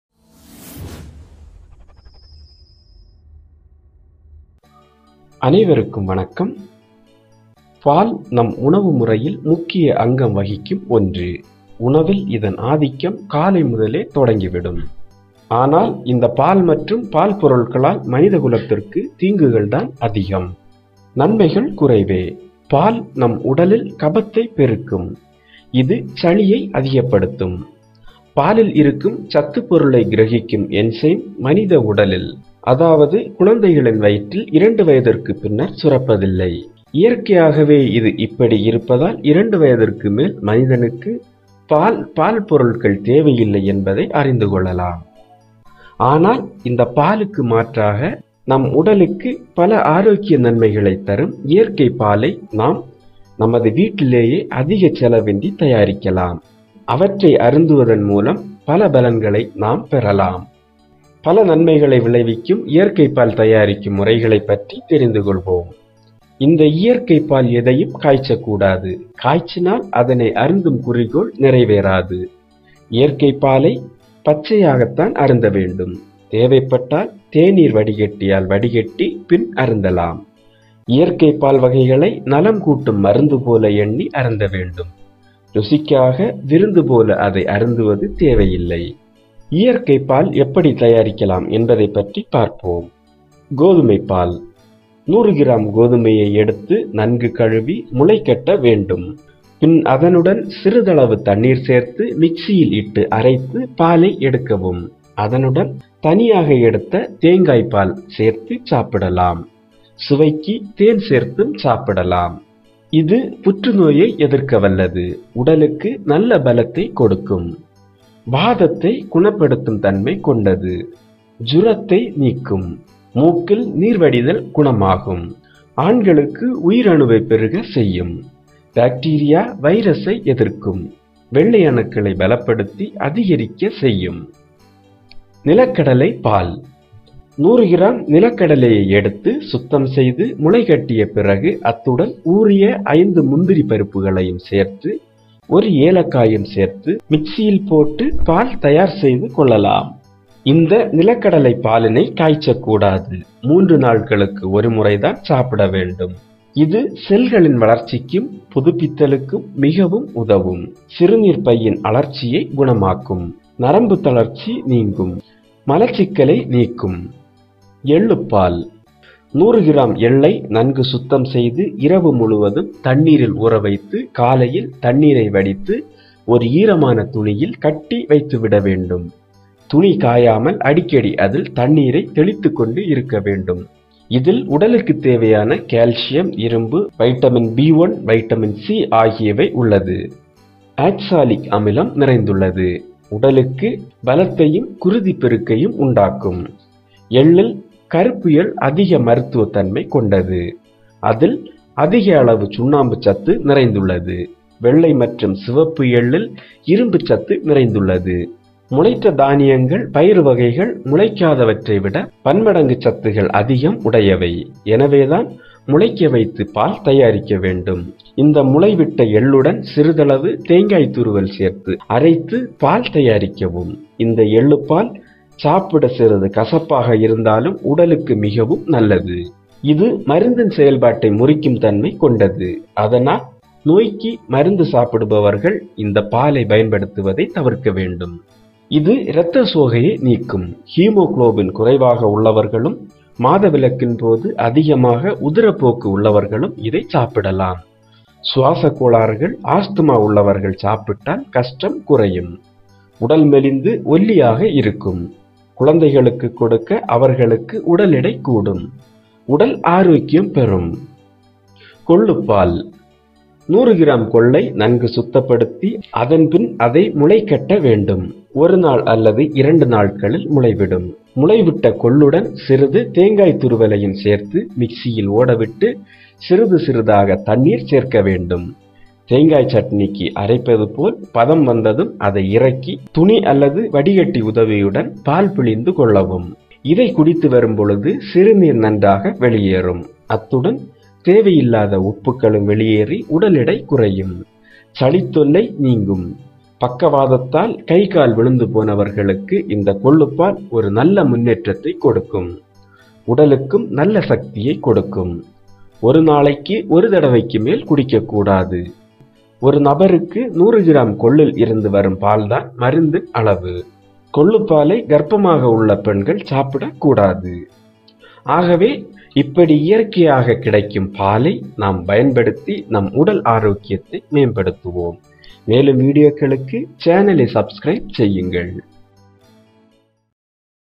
கோதுமை,நிலக்கடலை,எள்ளு போன்ற இயற்கை பால் உடலுக்கு தரும் அற்புத நன்மைகள்
கோதுமை,நிலக்கடலை,எள்ளு,கொள்ளு, போன்ற இயற்கை பால் நம் உடலுக்கு தரும் நன்மைகள் ஏராளம்.இப்பொழுது இதன் அருமையை உணர்ந்து பலரும் பயன்படுத்துகிறார்கள். இயற்கையோடு சார்ந்து வாழ இந்த உணவுகள் நமக்கு உதவும்.

iyarkai paal udalukku tharum arokiya nanmaikal parri therinthu kolvom

Tamil arokiya thagaval theritnhu kolvom